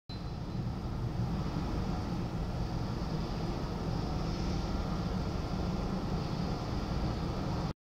DHL Airbus A300 Taxiing - London (EGLL)
An A300B4-622R |D-AEAC| taxiing to Runway 27L at London/Heathrow (EGLL/LHR). Sorry for the short upload, but there was another aircraft about to take off! This aircraft is wearing the Rugby World Cup Livery.